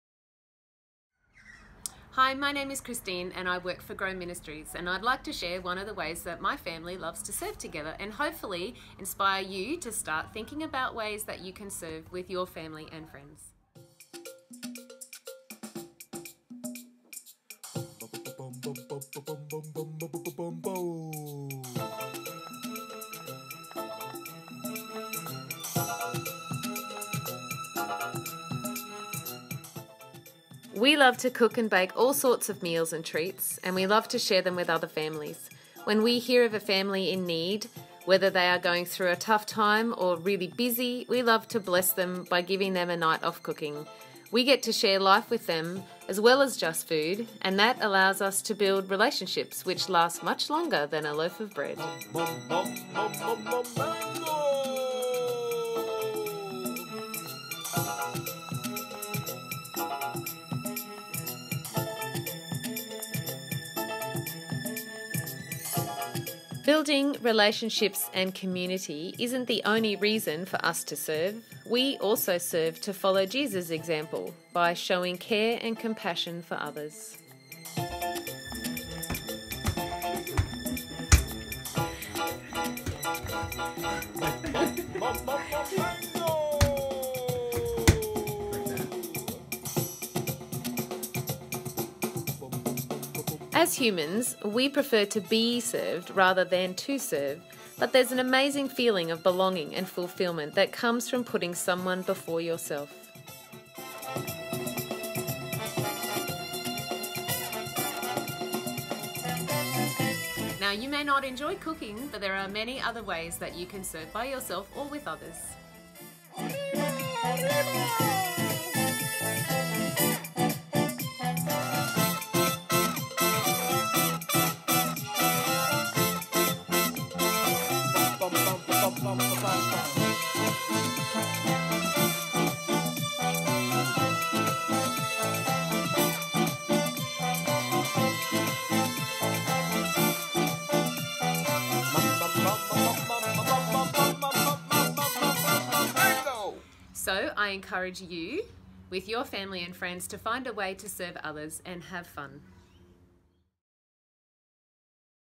Acts of Service - The Matthias Family
Jesus calls us to show love and serve one another. We serve because Jesus came ‘to serve and not be served’. Jesus’ example shows us that he was here to show care, concern and compassion for others and that it was done in love. Here is a story of a beautiful family that loves to serve!